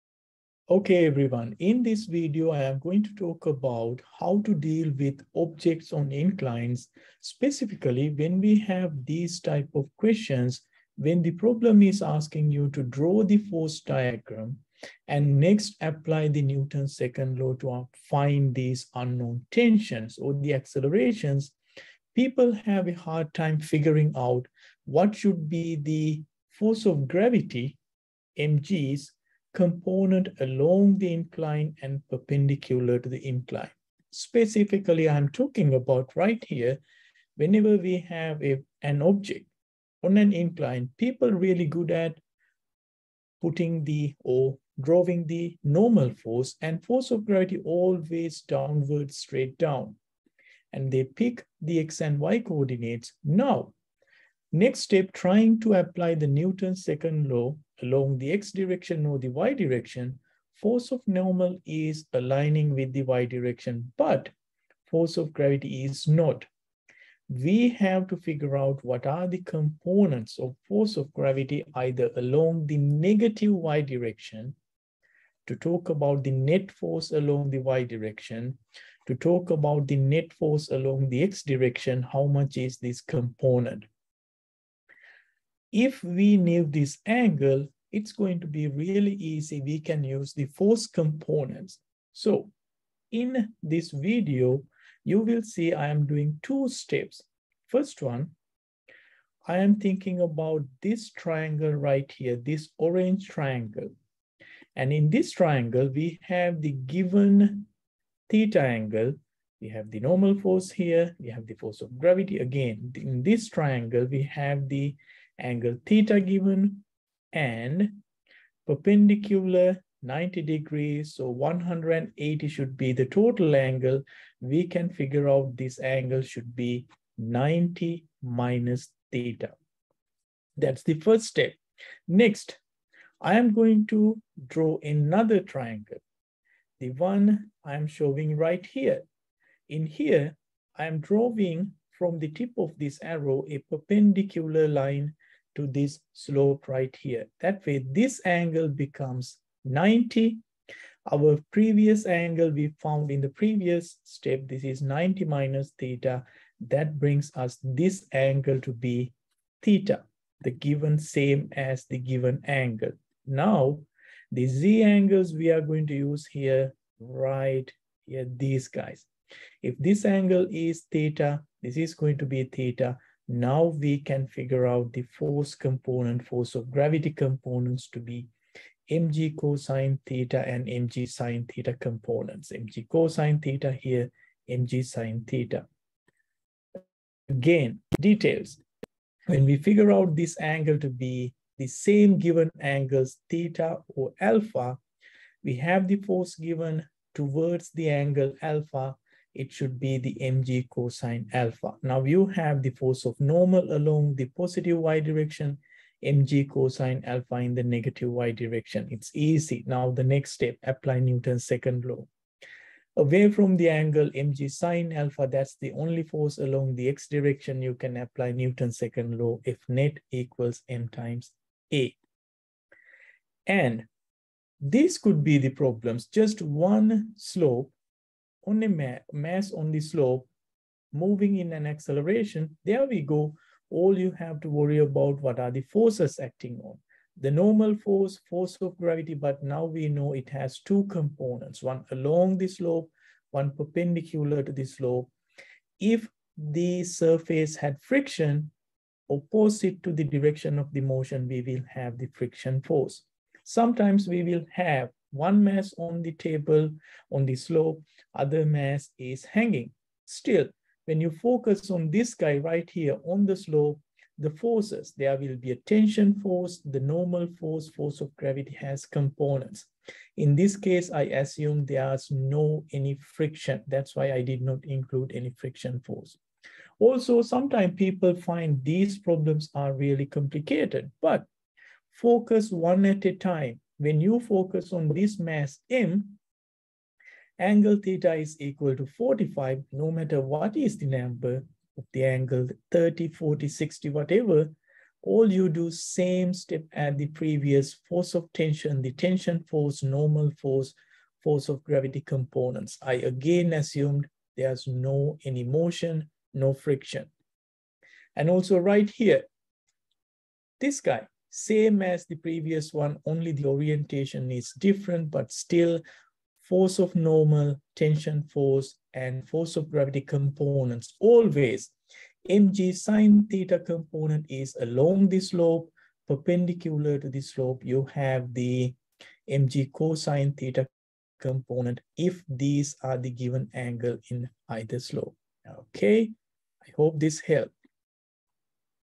Object on an incline- figuring out components of gravitational force and examples
calculus based physics: Object on an incline  -  figuring out components of gravitational force and examples